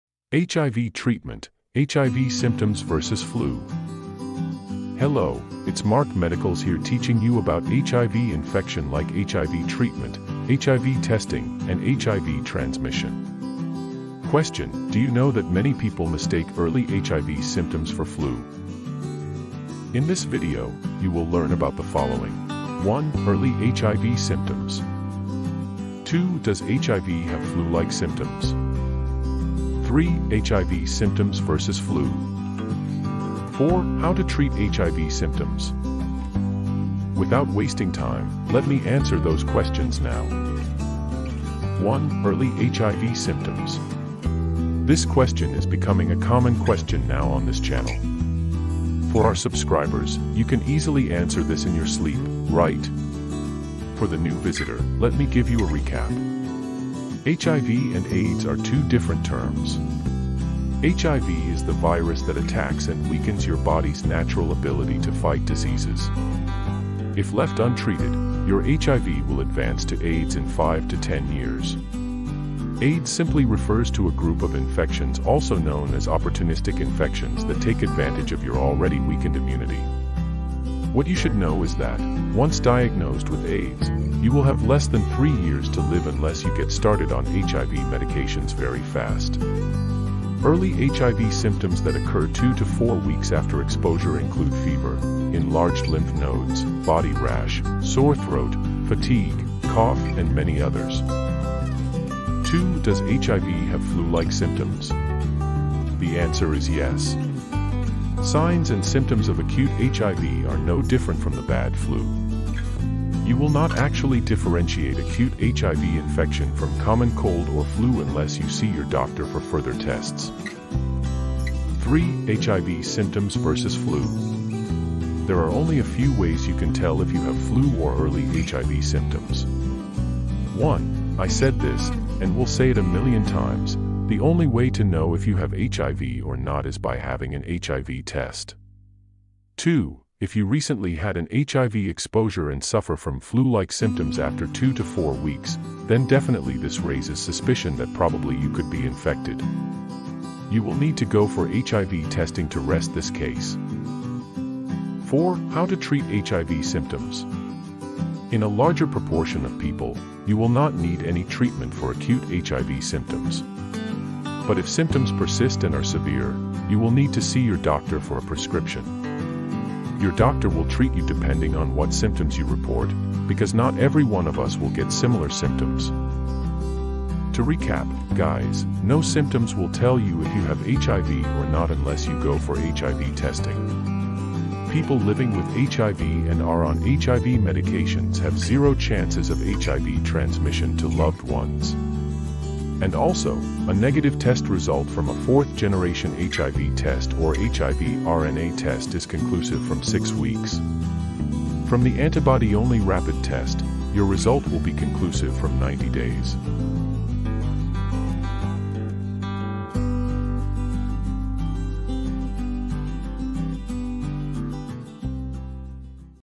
HIV symptoms Vs flu- the differences
hiv symptoms Vs flu:  There are only a few ways you can tell if you have flu or early hiv symptoms. 1: I said this, and will say it a million times, the only way to know if you have hiv or not is by having an hiv test. 2: If you recently had an hiv exposure and suffer from flu-like symptoms after 2 to 4 weeks, then definitely this raises suspicion that probably you could be infected. You will need to go for hiv testing to rest this case. In this video, you will learn about the following:
1. Early hiv symptoms.
2. does hiv have flu-like symptoms?
3. hiv symptoms Versus flu.
4. How to treat hiv symptoms?

TOP VIDEOS.